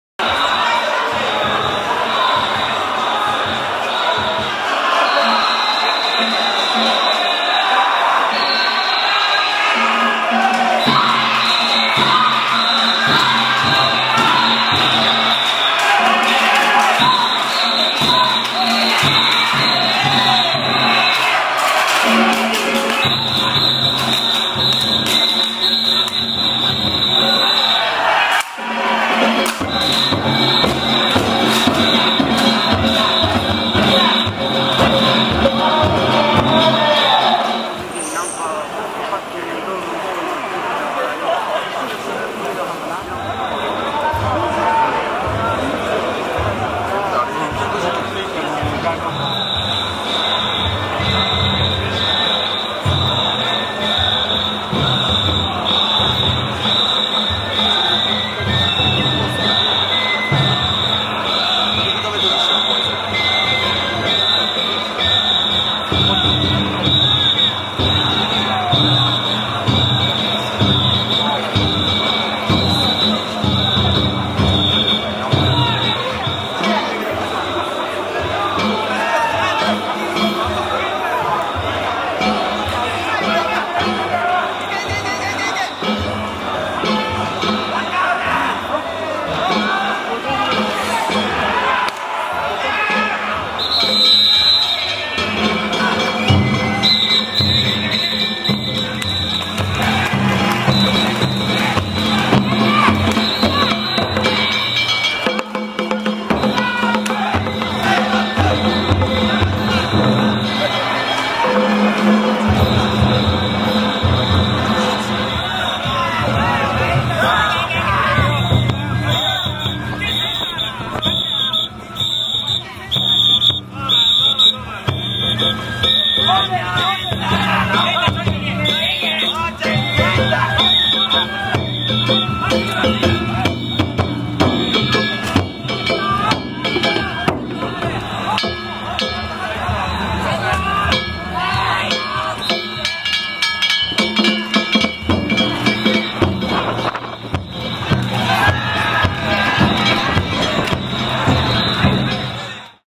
別所町 岸和田だんじり祭＿2022年
カンカン場、岸和田駅前通り商店街。 別所町の曳行光景。3本足のヤタカラスを彫り込んだまといが先導する。岸和田だんじり祭、2022年9月撮影。OMD-EM1mark2